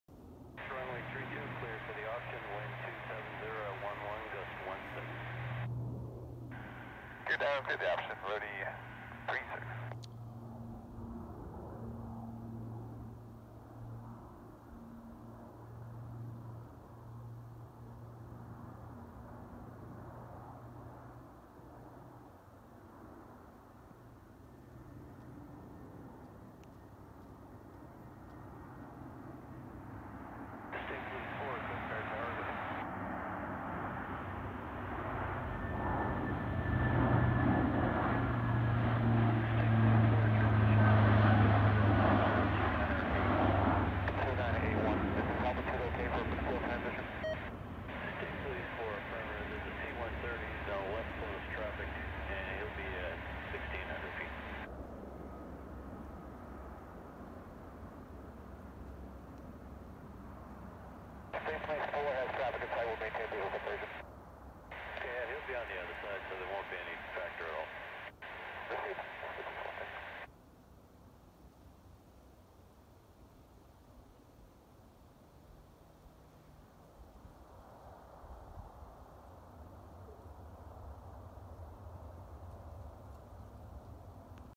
USAF Lockheed Martin C-130J Super Hercules Left-Close, Touch-and-Go Landing at Otis ANGB-1/8/20
I shot this United States Air Force Lockheed Martin C-130J Super Hercules (Callsign: RHODY36, Registration# - 05-1436, Serial# - 382-5575, Delivered: 2005) earlier this afternoon performing a left-close, touch-and-go landing onto Runway 32 at Camp Edwards/Coast Guard Air Station Cape Cod/Joint Base Cape Cod/Otis ANGB/Air National Guard Base in Bourne/Falmouth/Mashpee/Sandwich, Massachusetts. RHODY36 is a part of the 143rd Airlift Wing (i.e., AW) and 143rd Airlift Squadron (or, AS), which are both based at Quonset Point Air National Guard Station/Airport in North Kingstown, Rhode Island. Sorry. This was the best that I could do with all of the trees in the way as I was kind of out of position for this one.